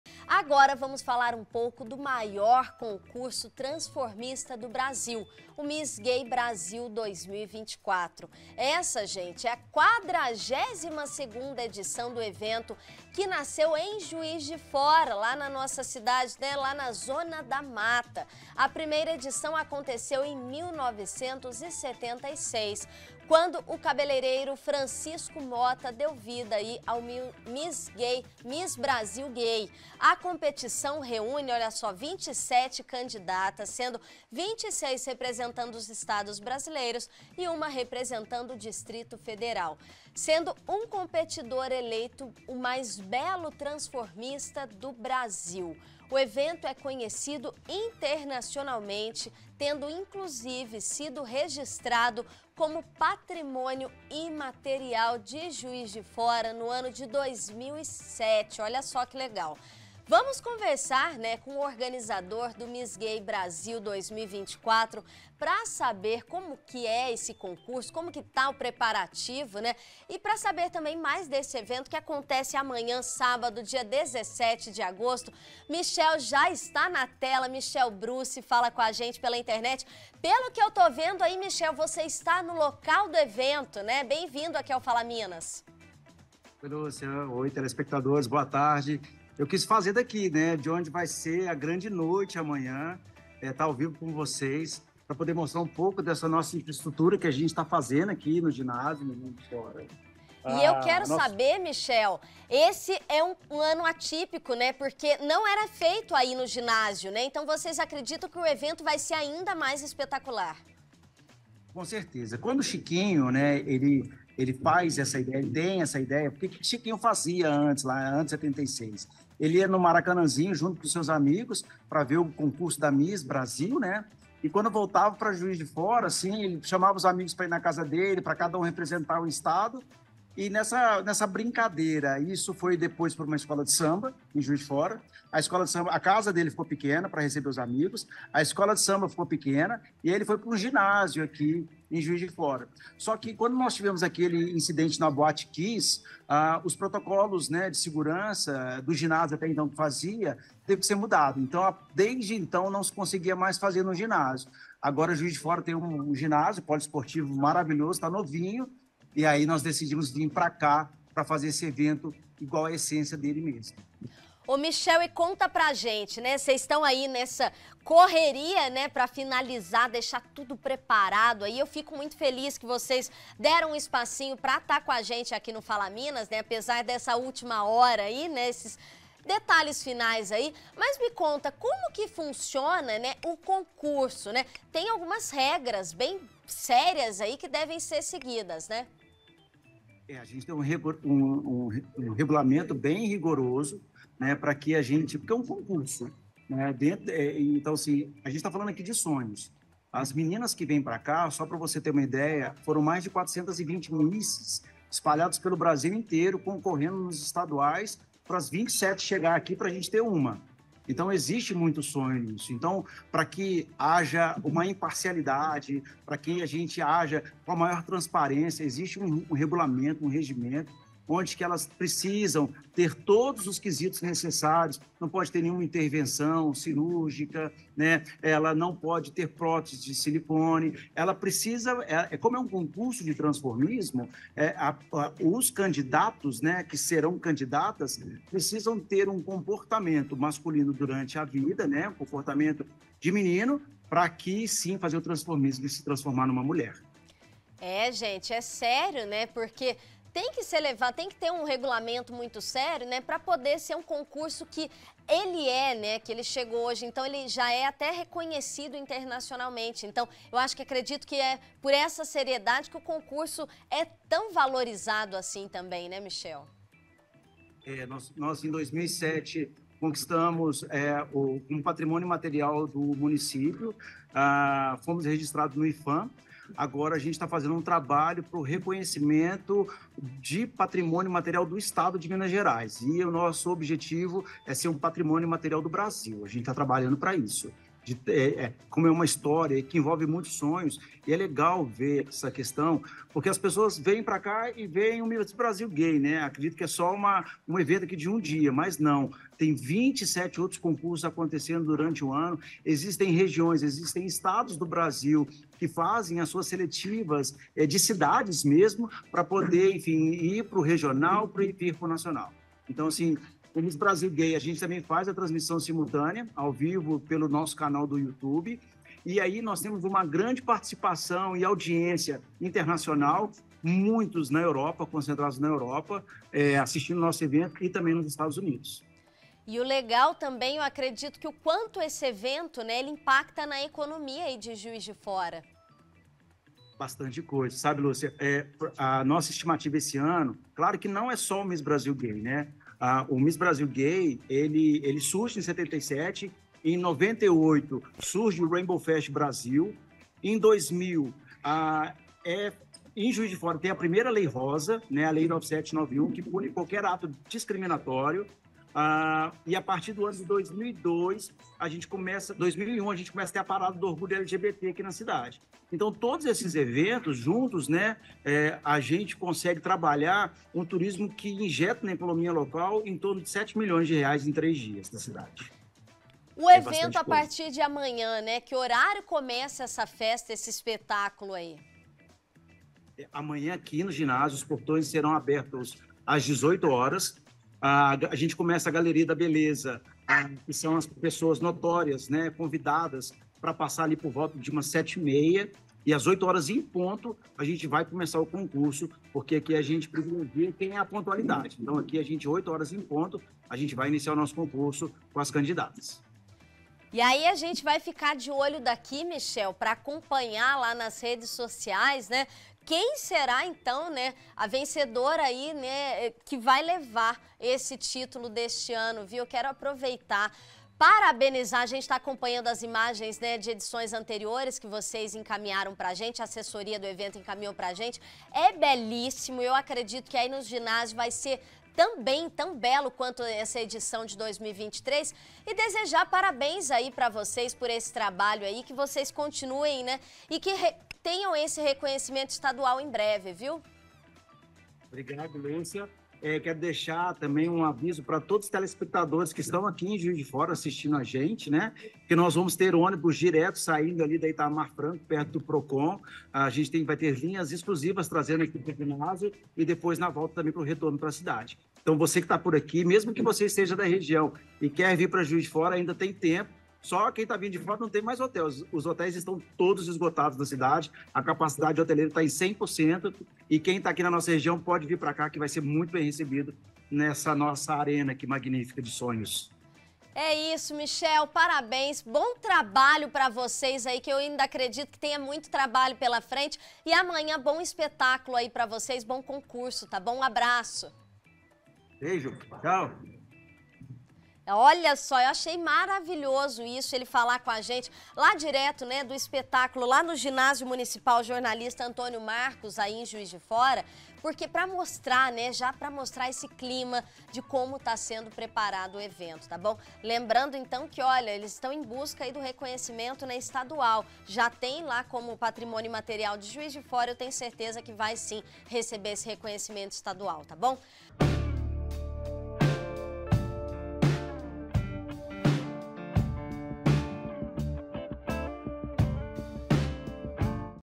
MISS BRASIL GAY 2024: JUIZ DE FORA SEDIA 42ª EDIÇÃO DO EVENTO
Juiz de Fora se prepara para receber a 42ª edição do Miss Brasil Gay.  A edição 2024 será neste sábado, a partir das 18h, no Ginásio Municipal Jornalista Antônio Marcos. Conversamos com o organizador Michel Brucce que, diretamente do ginásio, falou sobre a expectativa para o espetáculo.